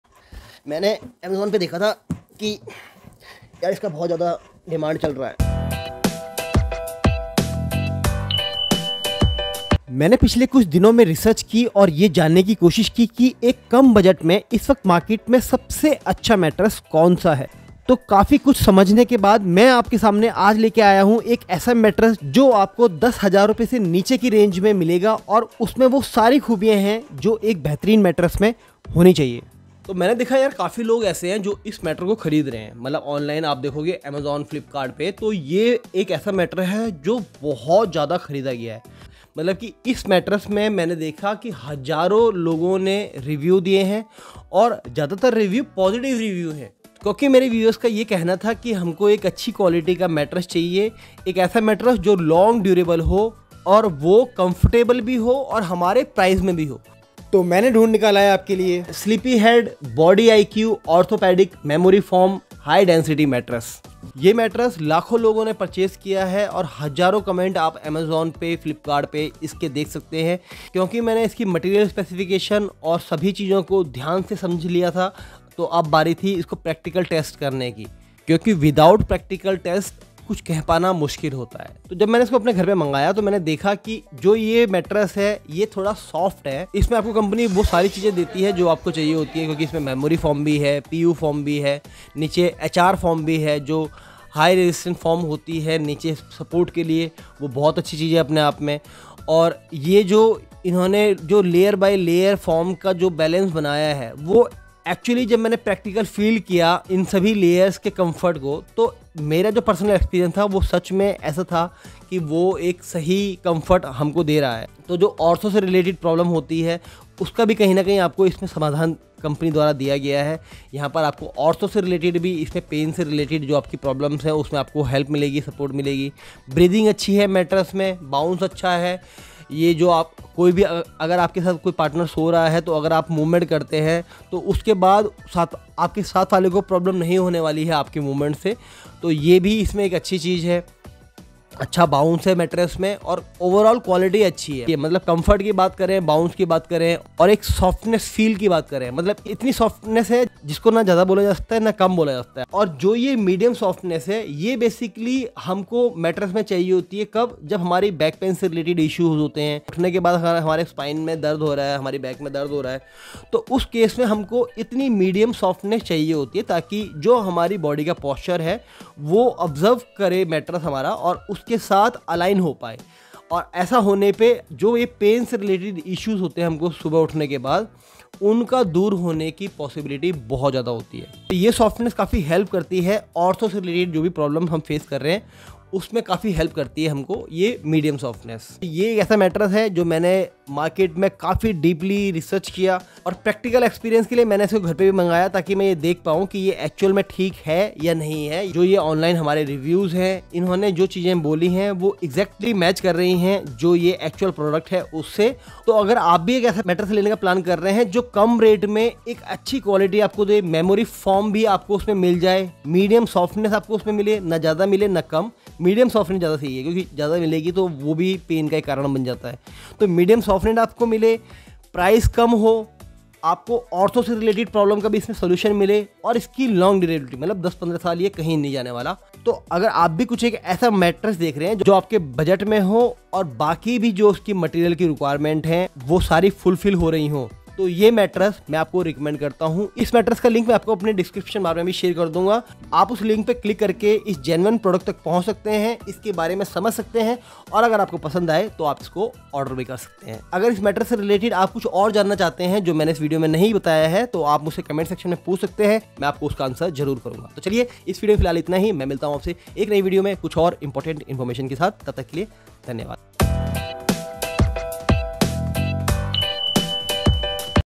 Best mattress below 10000 🤩 | Affordable Mattress Recommendation | Interior Clique
इस वीडियो में, मैं आपके लिए 10,000 रुपये से कम कीमत में सबसे बेहतरीन मैट्रेस लेकर आया हूँ। अगर आप एक बजट-फ्रेंडली और आरामदायक मैट्रेस की तलाश में हैं, तो यह वीडियो आपके लिए है। मैंने अपनी रिसर्च के आधार पर यह मैट्रेस रिकमेंड किया है, जो न सिर्फ किफायती है बल्कि बेहतरीन क्वालिटी भी प्रदान करता है।

🔗 **Amazon on Amazon**